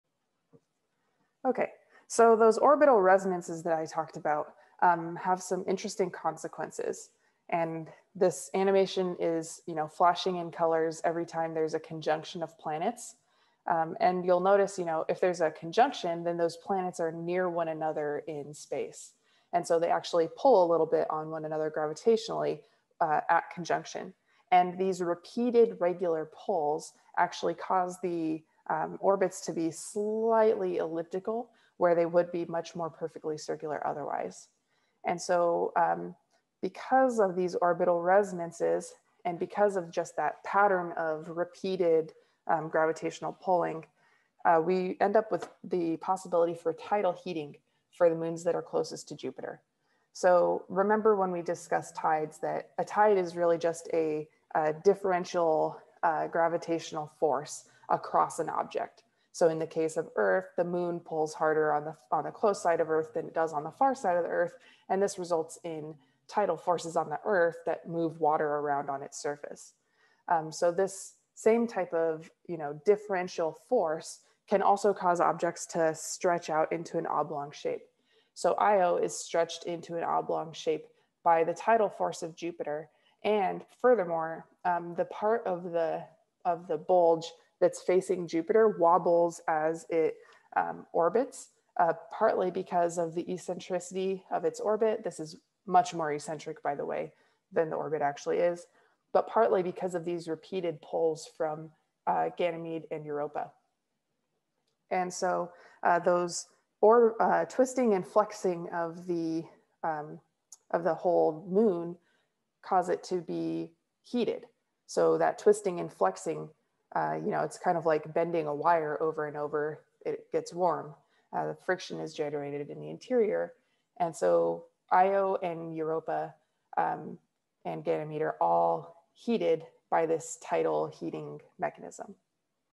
8-1-3 Tidal Heating of Moons
Where does the internal heat of moons come from?